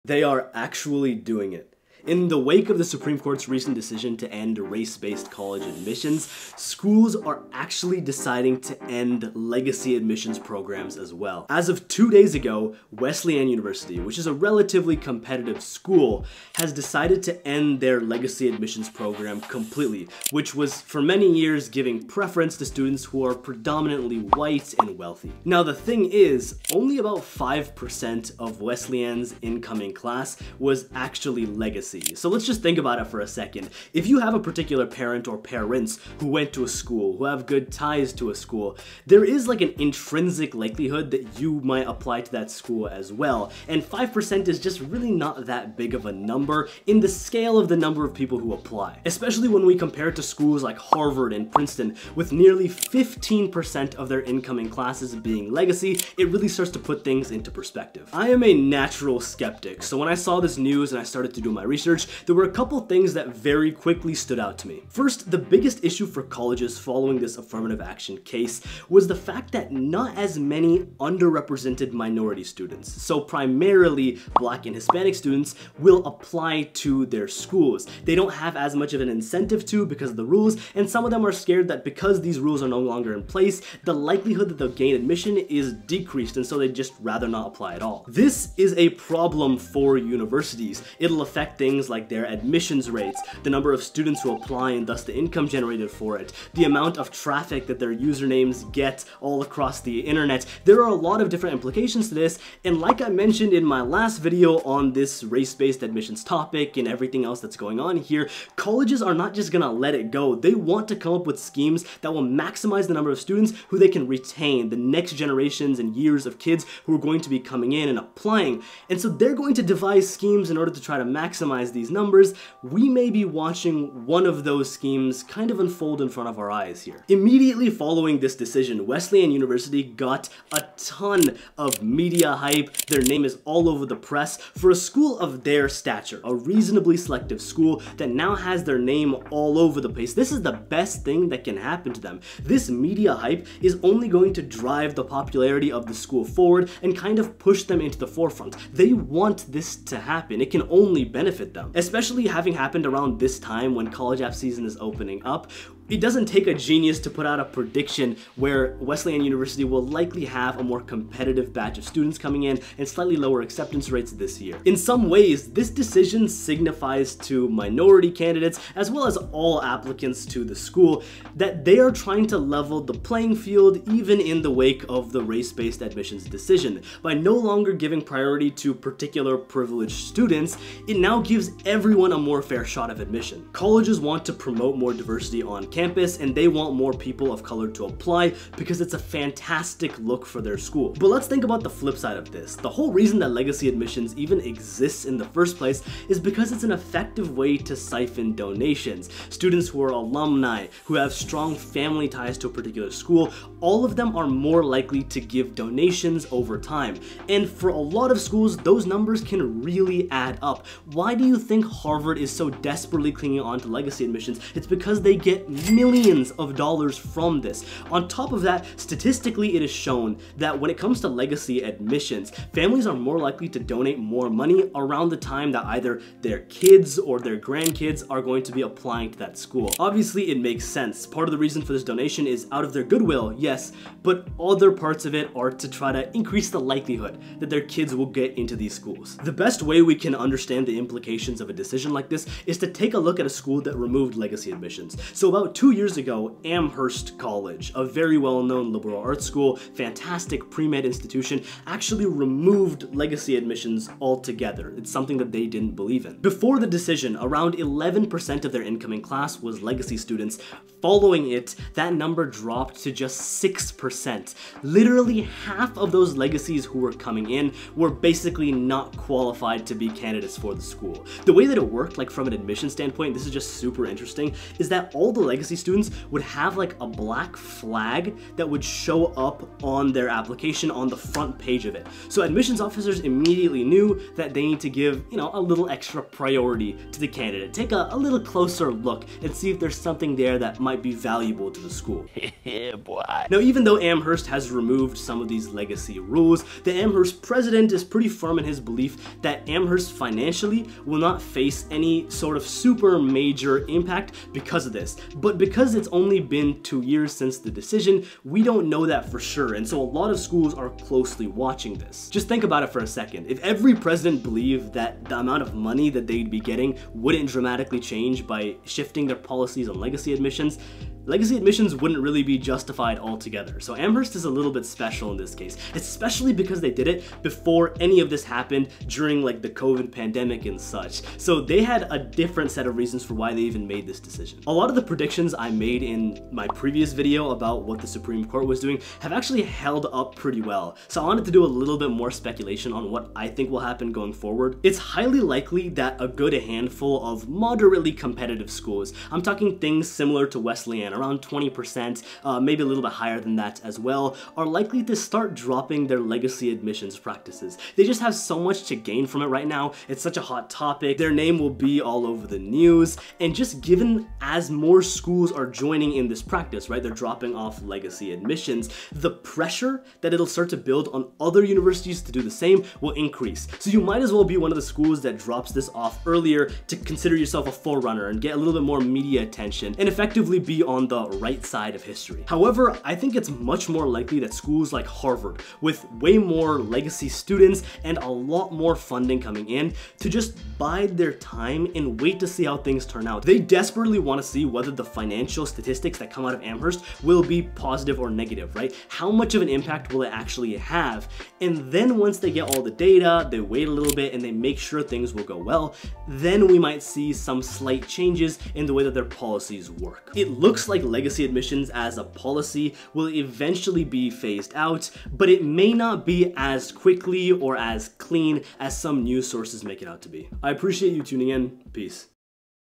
LEGACY ADMISSIONS IS OFFICIALLY ENDING
Interested in 1-on-1 consulting?

Wesleyan University recently made an unexpected decision to end legacy admissions. Let's talk about how this will impact college admissions moving forward.

Video edited and rendered by Cookie.exe.